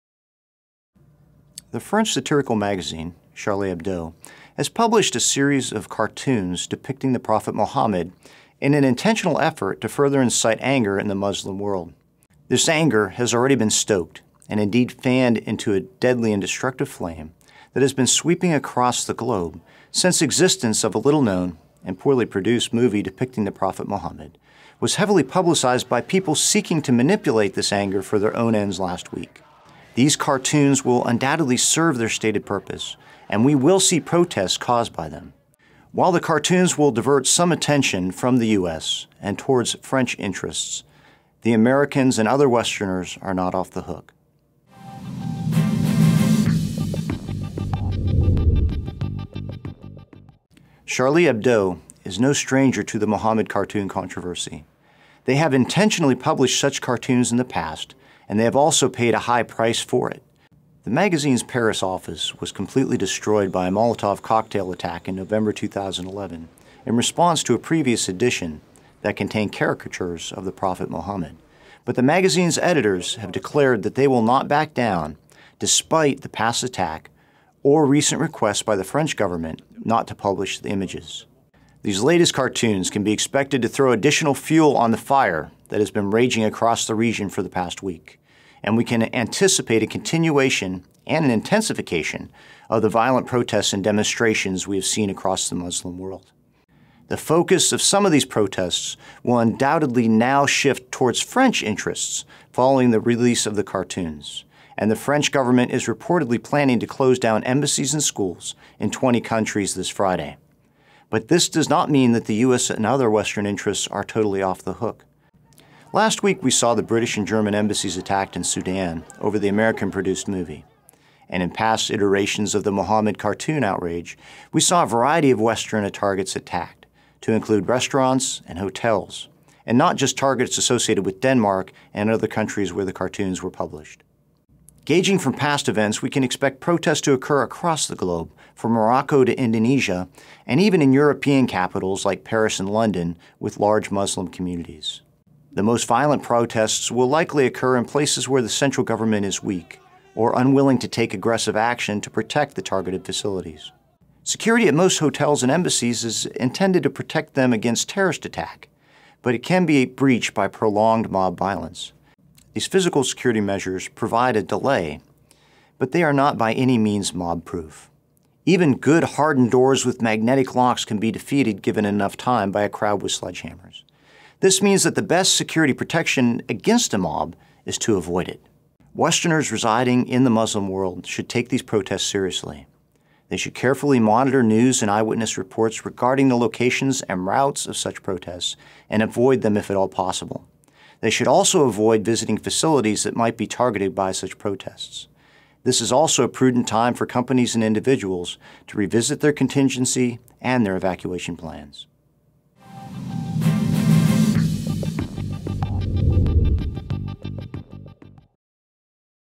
Prophet Mohammed Cartoons Published in France (Dispatch)
Stratfor's Vice President of Analysis Scott Stewart discusses a French magazine's recent publication of cartoons depicting the Prophet Mohammed and explains Western interests' need for heightened security measures in the Muslim world.